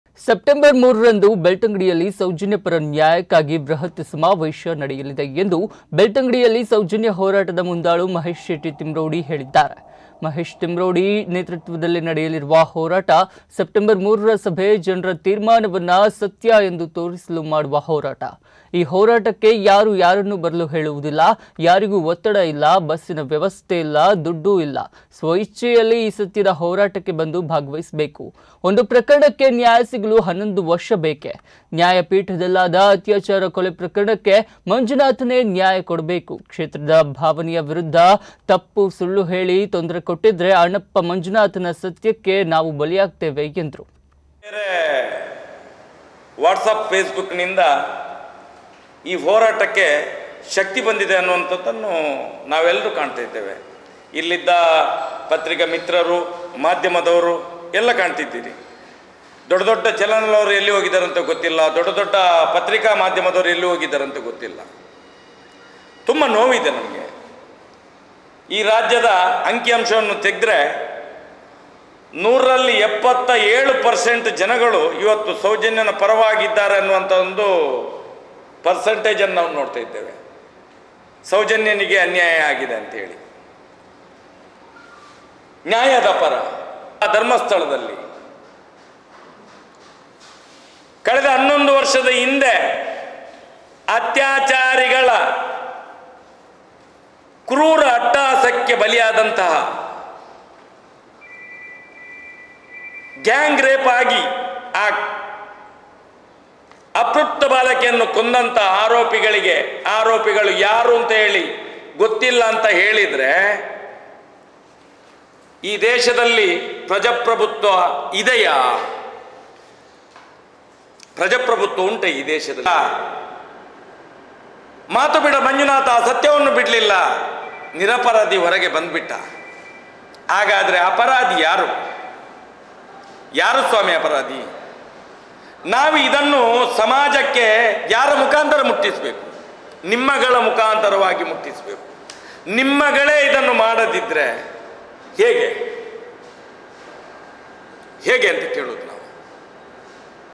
ಸೆಪ್ಟೆಂಬರ್ 3 ರಂದು ಬೆಳ್ತಂಗಡಿಯಲ್ಲಿ ಸೌಜನ್ಯ ಪರ ನ್ಯಾಯಕ್ಕಾಗಿ ಬೃಹತ್ ಸಮಾವೇಶ
ಬೆಳ್ತಂಗಡಿಯಲ್ಲಿ ಸೌಜನ್ಯ ಹೋರಾಟದ ಮುಂದಾಳು ಮಹೇಶ್ ಶೆಟ್ಟಿ ತಿಮರೋಡಿ ಹೇಳಿದ್ದಾರೆ
ಯಾರೂ ಯಾರನ್ನು ಬರಲು ಹೇಳುವುದಿಲ್ಲ,ಯಾರಿಗೂ ಒತ್ತಡ ಇಲ್ಲ, ಬಸ್ಸಿನ ವ್ಯವಸ್ಥೆ ಇಲ್ಲ, ದುಡ್ಡು ಇಲ್ಲಾ..!ಸ್ವಇಚ್ಛೆಯಲ್ಲಿ ಈ ಸತ್ಯದ ಹೋರಾಟ